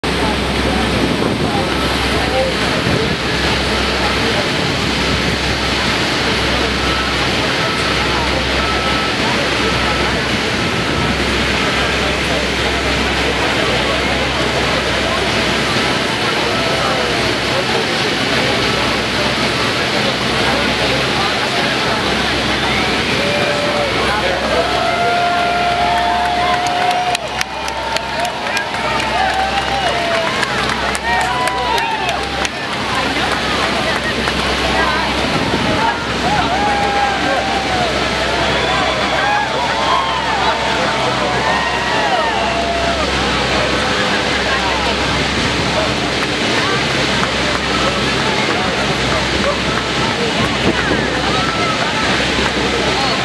Space Shuttle almost Hits a Light
Endeavour, turning from Sepulveda to Machchester, the orbiter misses a signal by inches! Taken during the move from Los Angeles International Airport to the Science center.